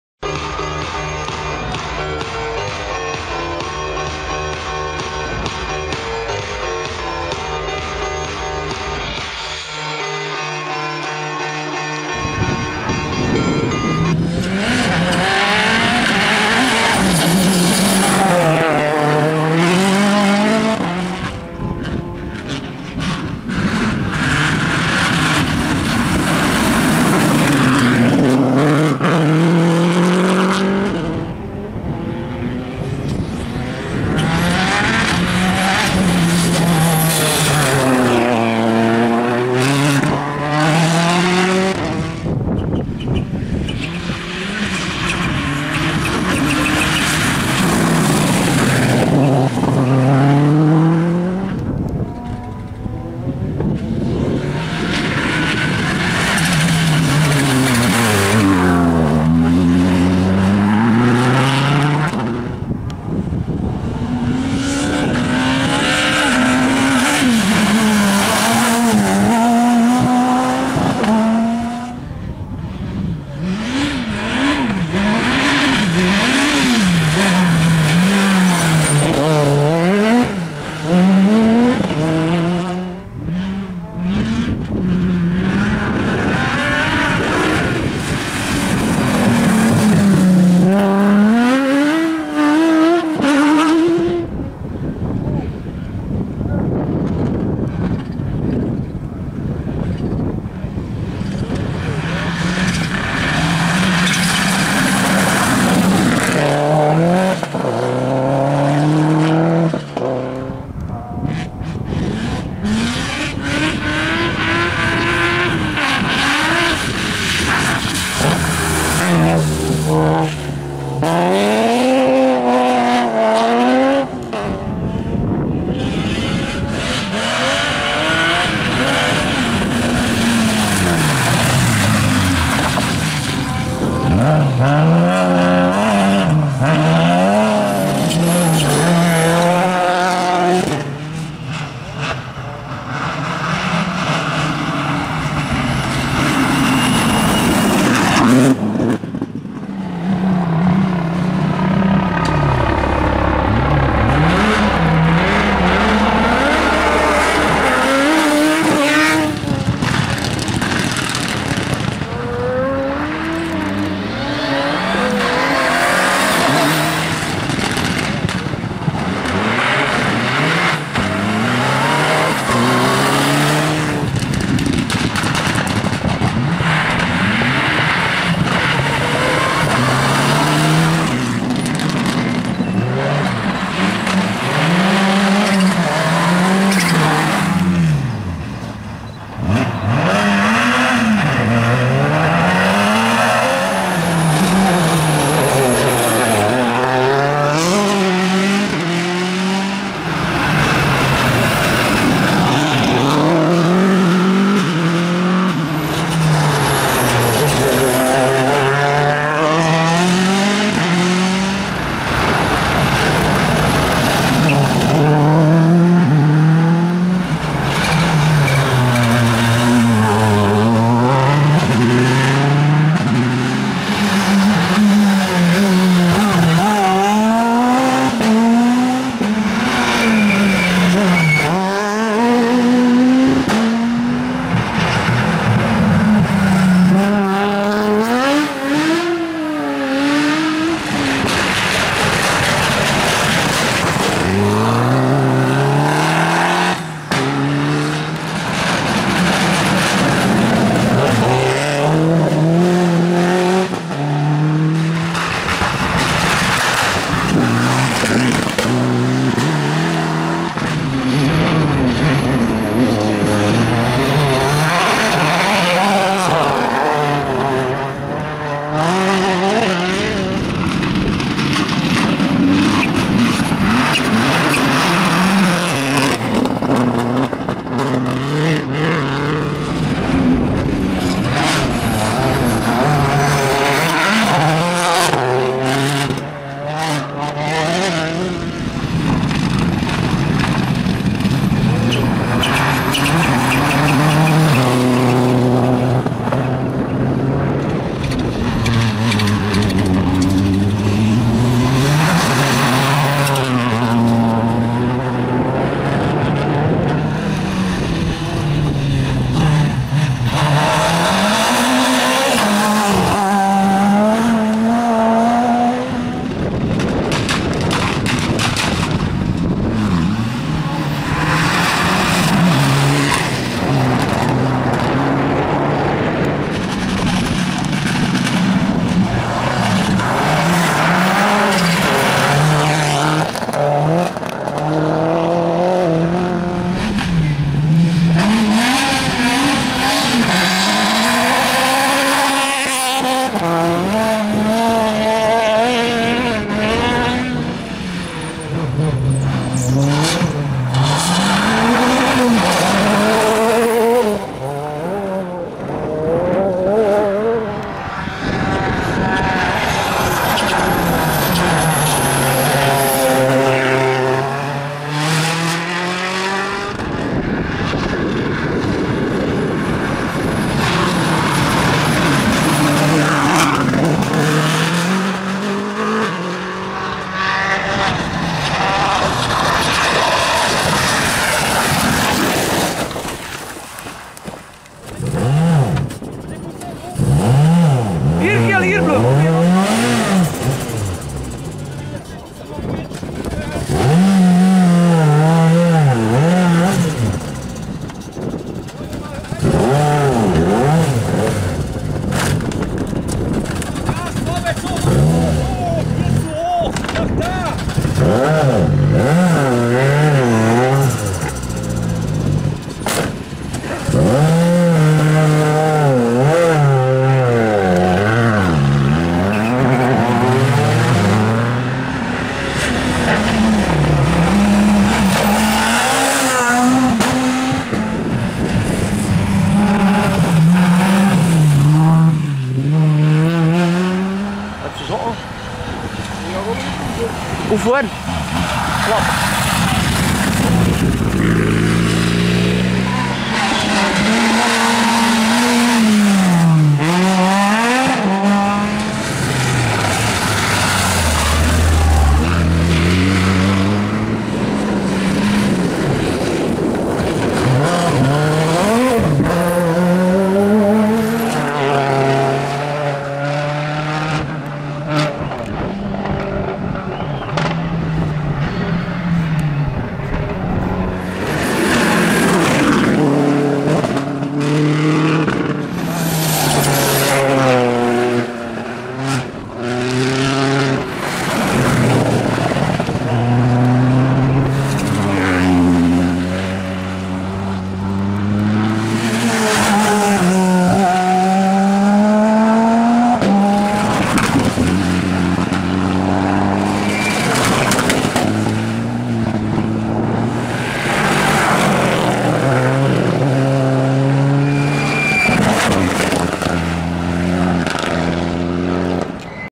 Haspengouw Rally 2011 Moderne
beelden van de moderne wagens in een vettige en natte Haspengouwrally 2011 , beelden zijn gemaakt door Kjell Maveau , meer filmpjes op